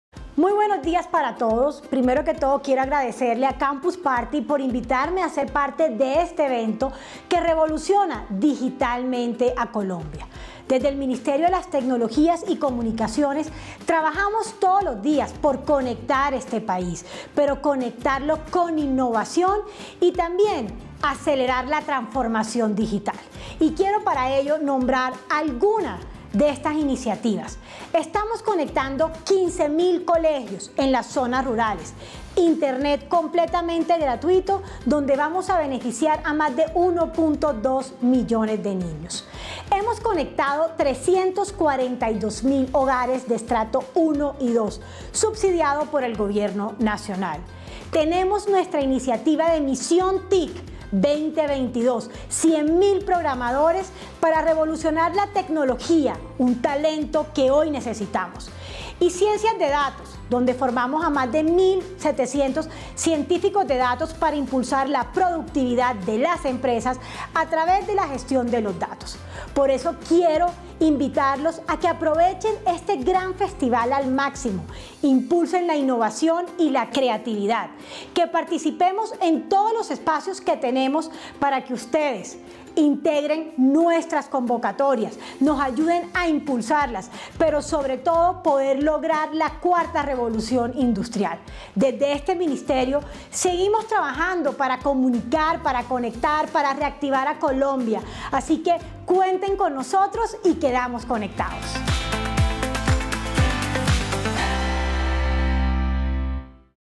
MinTICEnCampus | La ministra Karen Abudinen trae buenas noticias sobre innovación a Campus Party
En el marco de la décima edición de Campus Party, la ministra Karen Abudinen presenta los avances en materia de conectividad, transformación digital e innovación para toda Colombia.